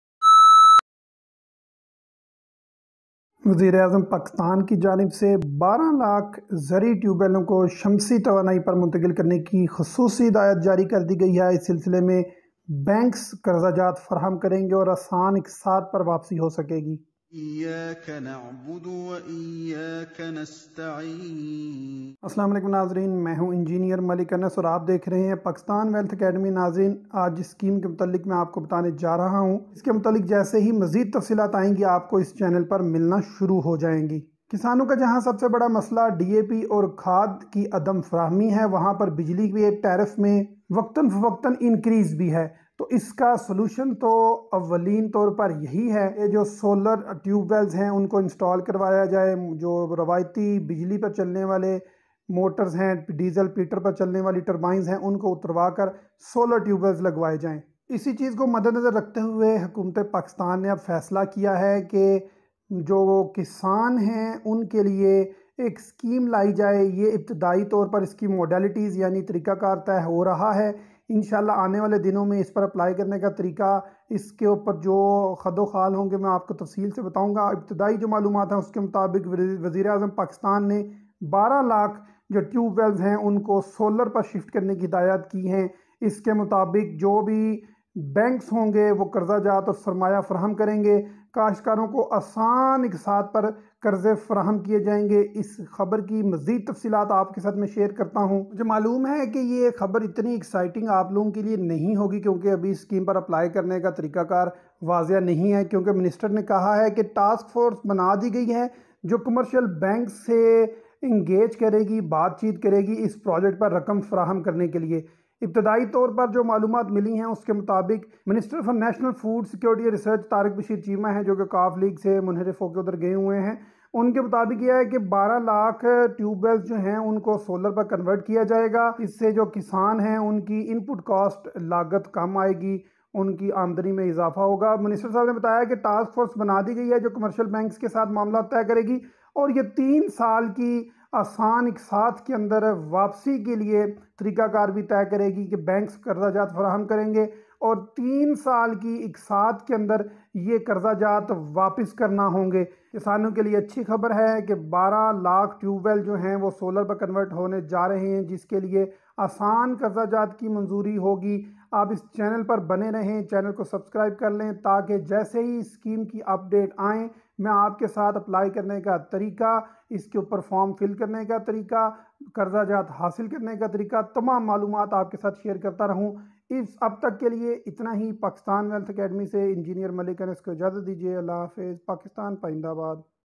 Solar tubewell scheme for Farmers on easy installment
Minister for National Food Security and Research Tariq Bashir Cheema said on Tuesday that over 1.2 million agriculture tube wells would be switched to solar energy to reduce input costs and enhance farmers’ income.

Addressing a press conference, the minister said that a task force was being formed to finalise modalities and engage commercial banks to finance the project. He said that under the facility, the farmers would have to repay the loans in instalments in three years.

He said that the initiative has been taken on the directives of Prime Minister Shehbaz Sharif, who has also asked for formulating short and long-term policies to ensure tangible results in the shortest possible time with a focus on reviving the agriculture sector to ensure food security as well as the prosperity of farmers.

While reaffirming the government's commitment to ensuring a proper rate of return to the farmers for their produces as well as enhancing the output of all major and minor crops, he said that recommendations for cotton intervention price had been finalised and will be taken up at the next meeting of the federal cabinet for approval.

The minister said that the initiative aimed at encouraging the farmers to bring maximum areas under cultivation as the cultivation of crops during the last season reduced by 37 per cent, which was replaced by rice, maize and sugarcane.

The minister said that the government was also keen to announce a minimum support price for wheat before the start of the sowing season to encourage farmers to sow more wheat for attaining self-sufficiency in staple grain production and reduce reliance on imported wheat and edible oil that were eating up foreign exchange reserves.

He said that the government was aware of rising inflation and the problems being faced by the common man and that steps were taken to protect them from the inflationary pressures.

He said that the government through the Utility Stores Corporation outlets was ensuring the provision of essential commodities at subsidised rates.

Cheema said that to avert any possible shortage of food grains owing to the ongoing Russia-Ukraine conflict, the government has beefed up its strategic reserves and enhanced wheat procurement targets of all provincial governments and PASSCO. He said that it has been decided to import about three million tons of wheat to ensure smooth supply throughout the year.

Highlighting the relief measures introduced by the government for the agriculture sector, he said that sales tax on tractors has been withdrawn, whereas seed cotton and canola had been exempted from sales tax and no tax has been levied on fertilisers to facilitate the growers.

The minister said that that the government was taking steps to ensure the availability of quality seeds and pesticides and has finalised negotiations with Monsanto company while work on the agriculture cooperation under the second phase of the China-Pakistan Economic Corridor project in livestock and research has also been also finalised. He said that a special task force was being formed to take the input of all the stakeholders to make the agriculture sector more viable and stronger